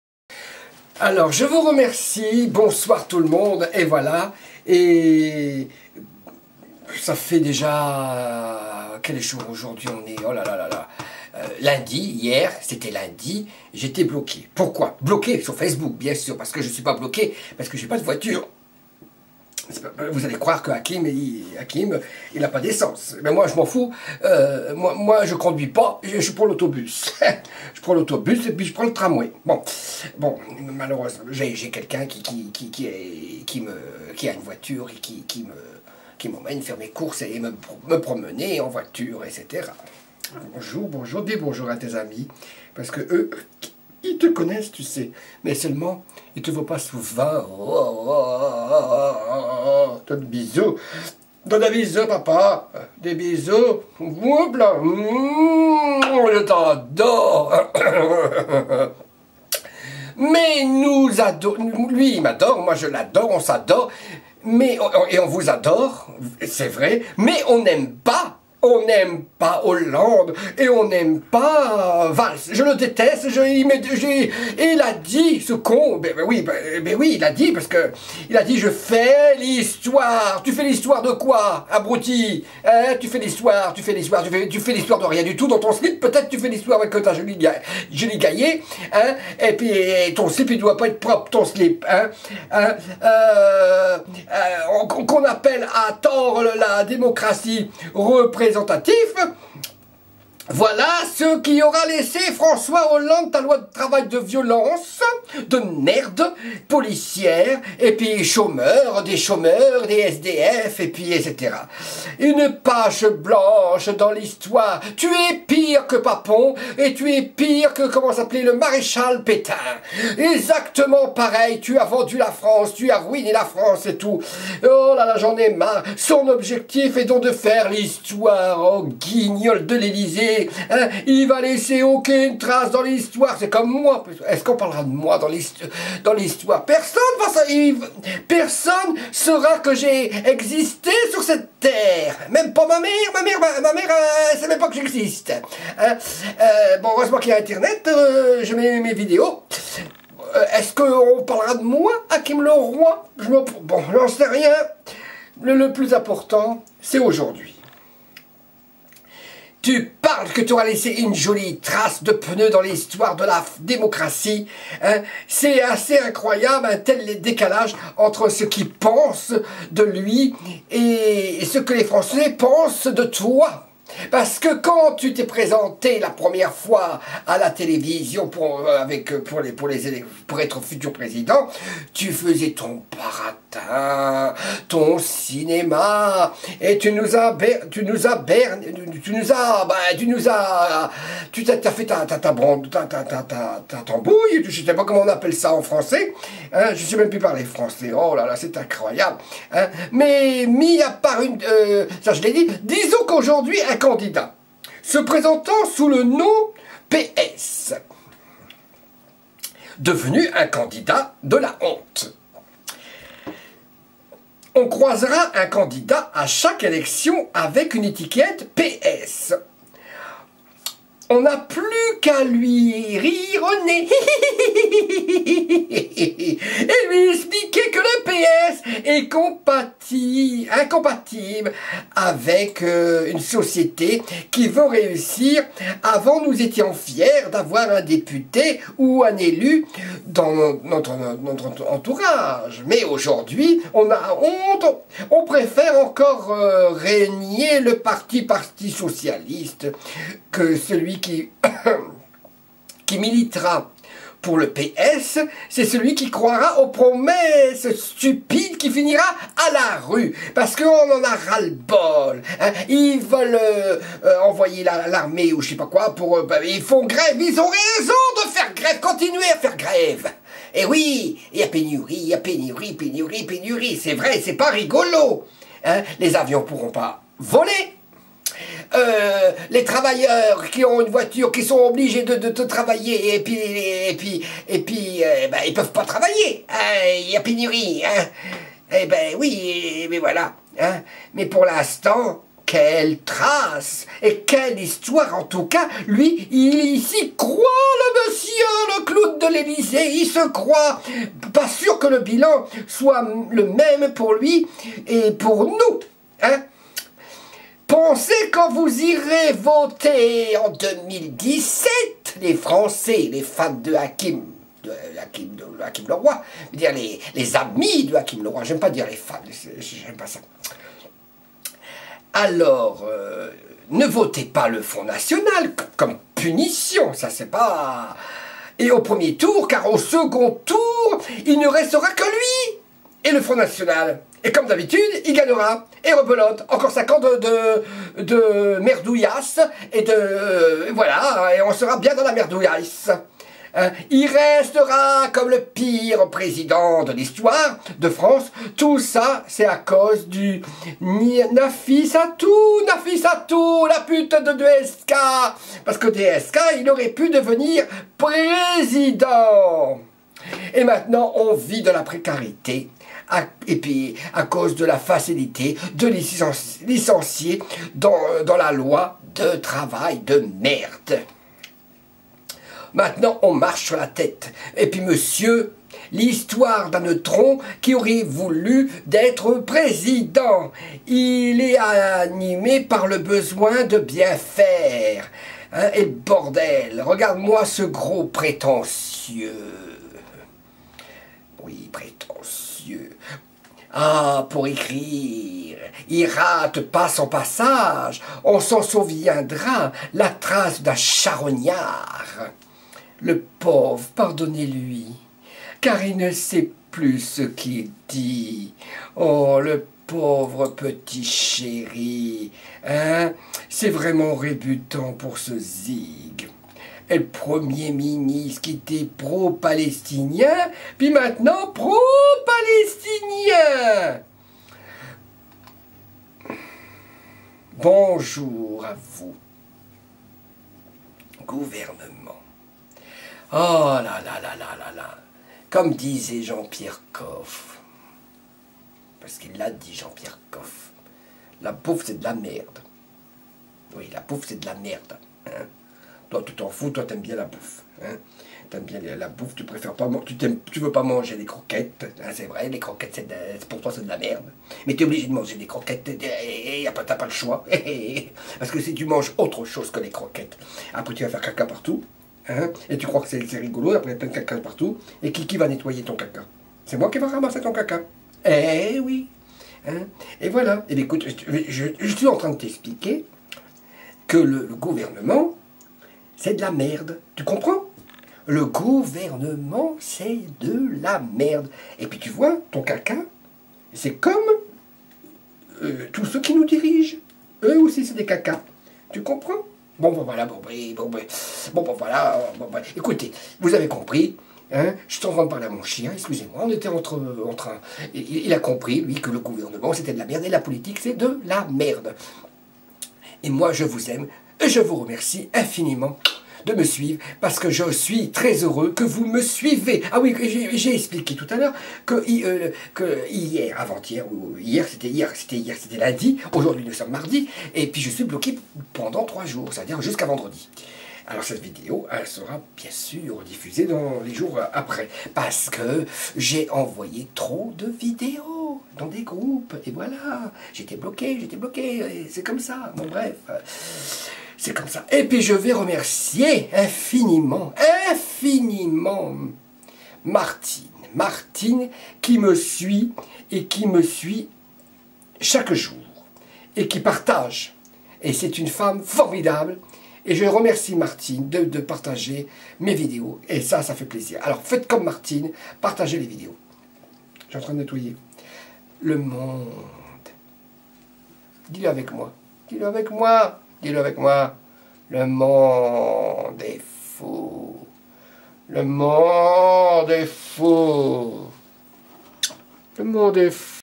videos no ,, 1805 francois hollande je fais l,histoire
mon cul il fais l.histoire ruiner la france champion le pire des pires des presidents , on regrette Mitterrand et Chirac dire que le la france a les pieds dans eau , et les manifestations et les gréves ces pas joyeux  ,ces bien triste tout cela ,le monde et fou ,
encore 11 mois après CA seras le déluge